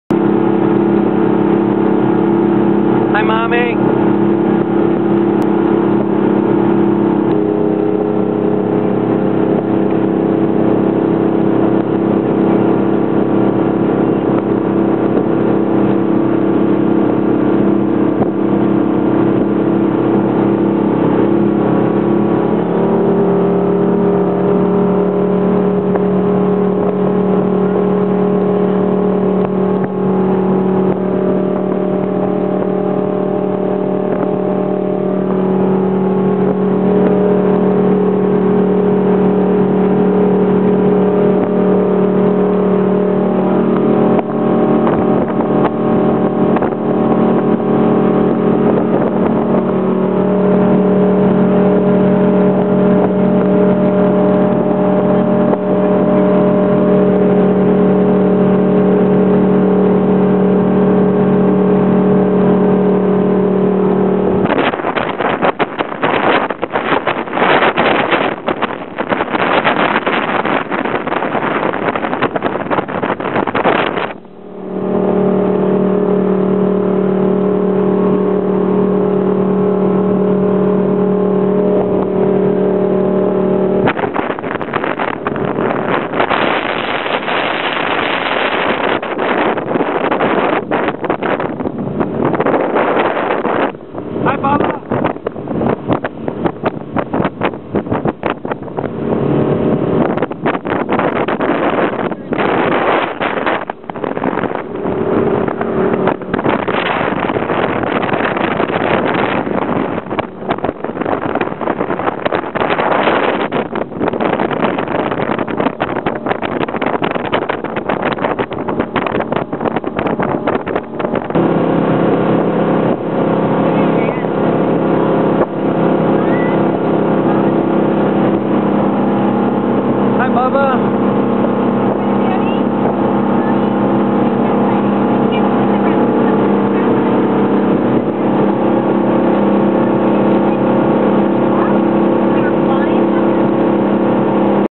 Old-Skool 2006 going 41kt
Out on the Potomac back in 2006, just a quick viddy of how it used to be.  Going 41kt (almost 47MPH) with plenty of throttle to spare, the old boat was a hoot! Near Quantico (I think), headed north and back to our old marina (Columbia Island Marina, DC)